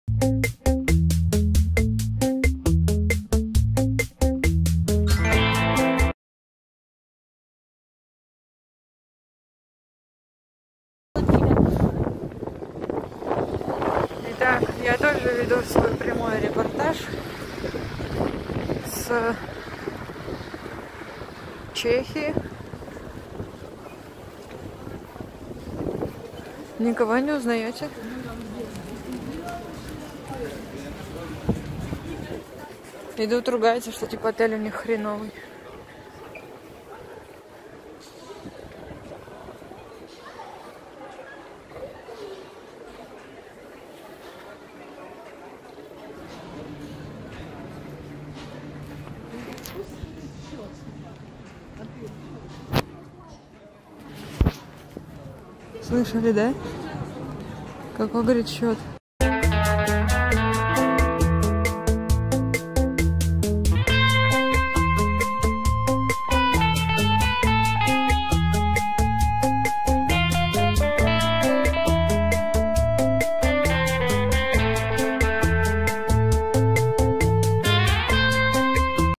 ГУСЕВА И РОМАНЕЦ ВСТРЕТИЛИ В ПРАГЕ (ondom2.com)
В этом выпуске новости и слухи  про Антона Гусева и Викторию Романец.